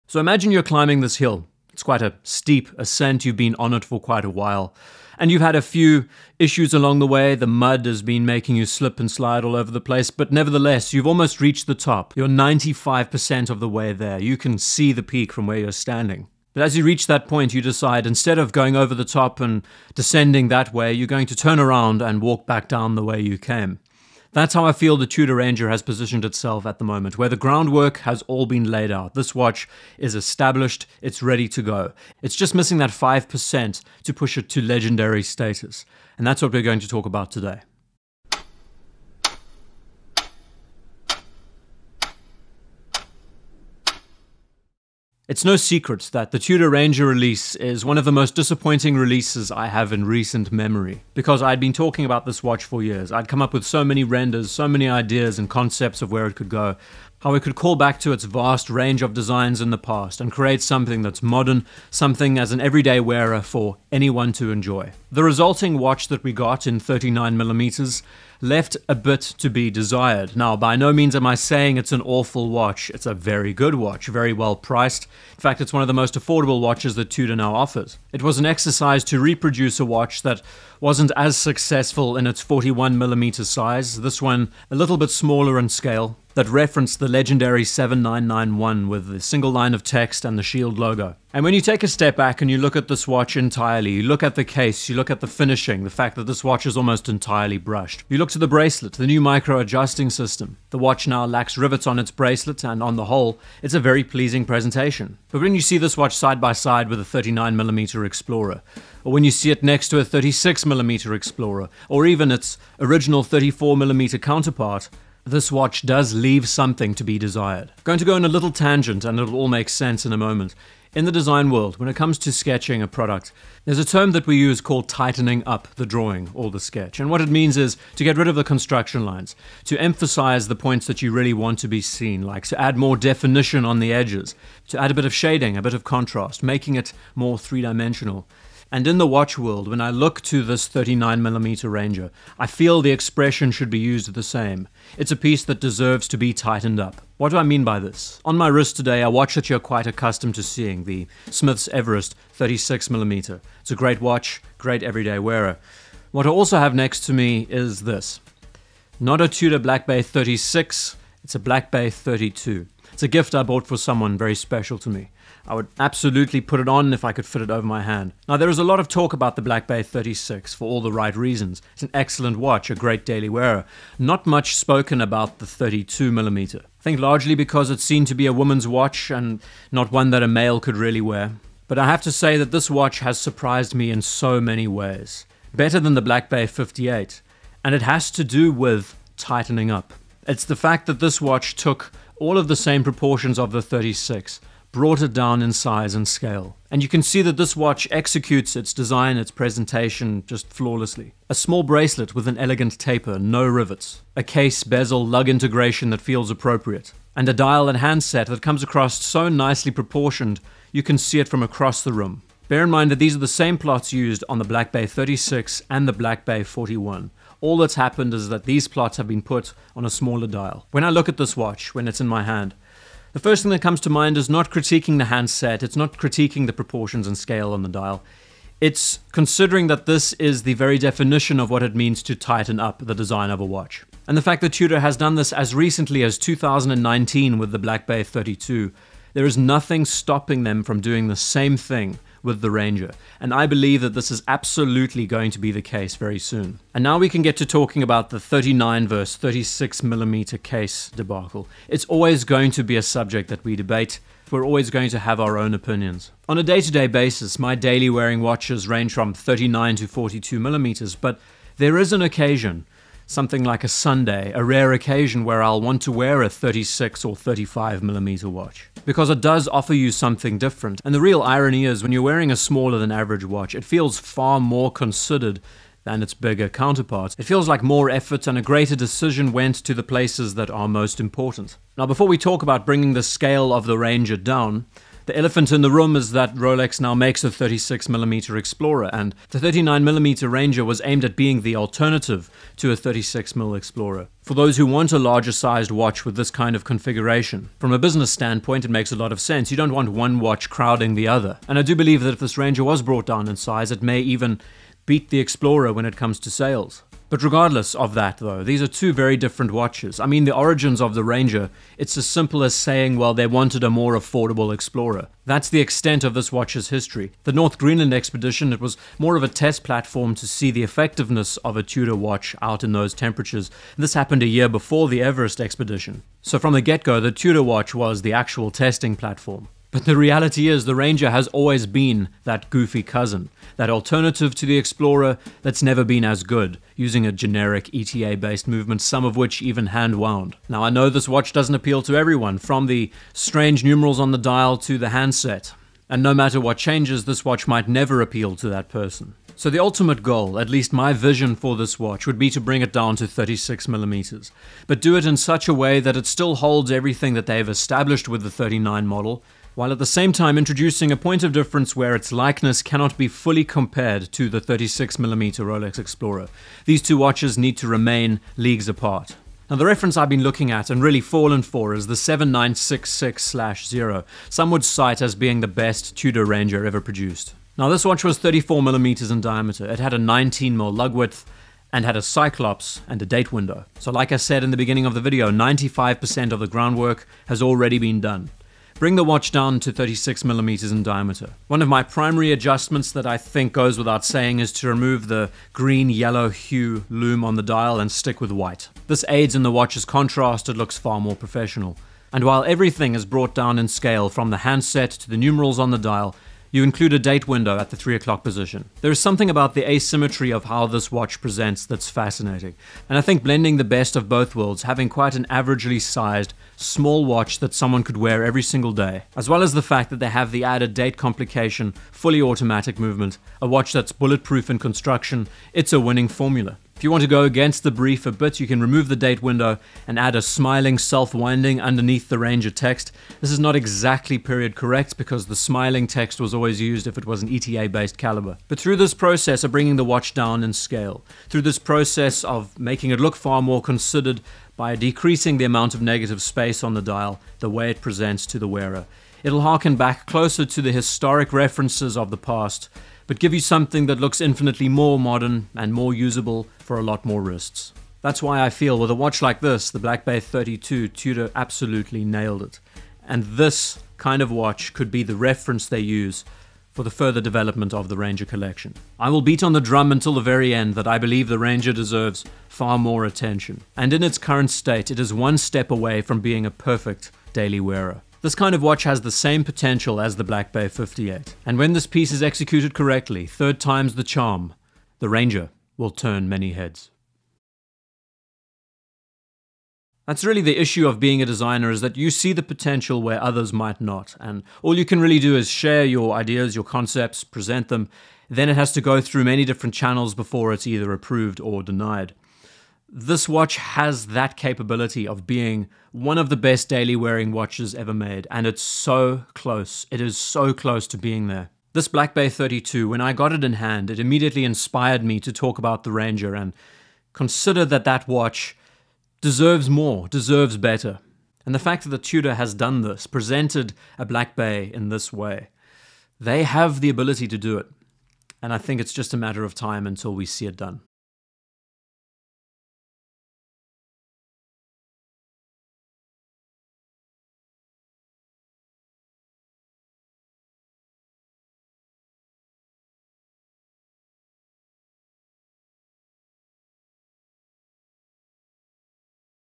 Here's How Tudor Could Perfect the Ranger (36mm Redesign)
A Discussion around the Tudor Ranger and how this watch has the potential of the being the perfect every day wearer. Taking the modern 39mm Ranger, shifting it down to 36mm, adding a date complication and including a better contrast of black and white on the dial. The resulting watch would fit a wider range of wrists and through this exercise of tightening up its design, the watch would come across feeling more complete.

Intro 0:00
Tightening Up a Design 1:46
On The Wrist & Black Bay 32 2:27
39mm vs 36mm 4:07
The Tudor Ranger Origins 5:07
36mm Ranger Redesign 5:49
Conclusion 7:49

All Images are Sourced from either Press Release Photos or Public Listings.
Below are a list of the common sources that are used in no particular order: